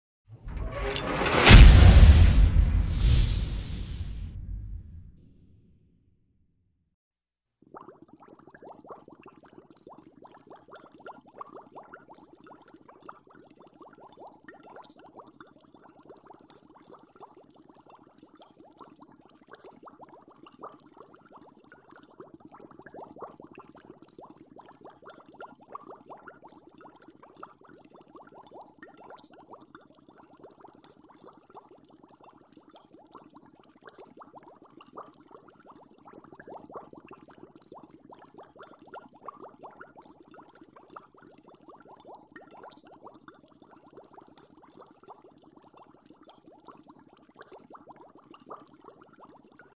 Efectos de Agua hirviendo 01 / Boiling Water Effect 01
Efecto de sonido de Agua hirviendo 01 / Boiling Water Sound Effect 01

Tendremos mas de 1000 efectos para tus proyectos / We will have more than 1000 effects for your projects